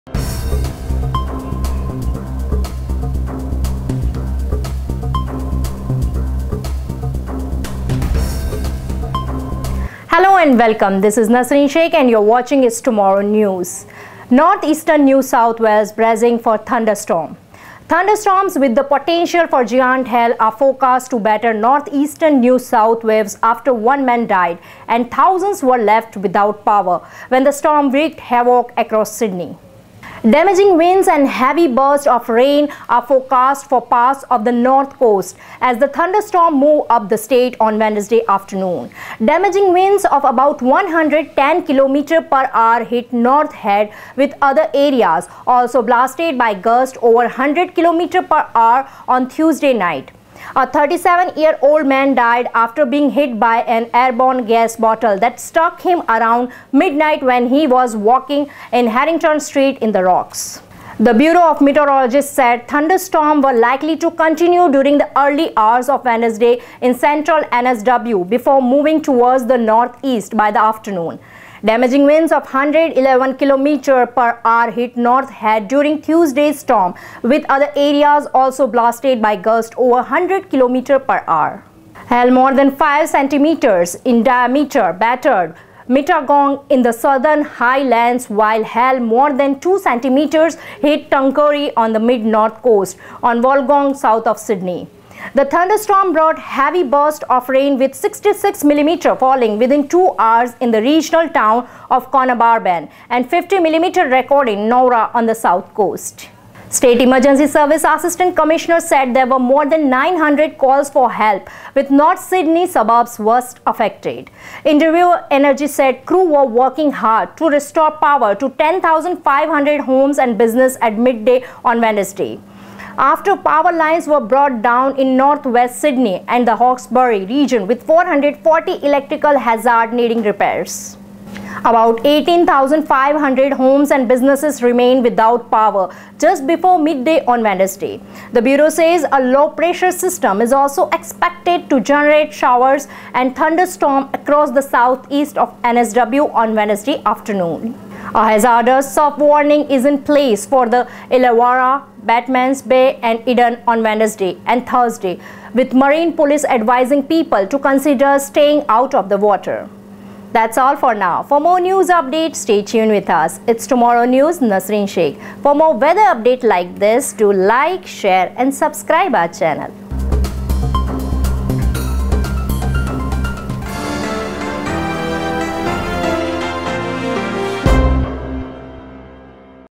Northeastern New South Wales bracing for thunderstorms
Thunderstorms with the potential for giant hail are forecast to batter northeastern New South Wales after one man died and thousands were left without power when the storms wreaked havoc across Sydney. Damaging winds and heavy bursts of rain are forecast for parts of the north coast as the thunderstorms move up the state on Wednesday afternoon.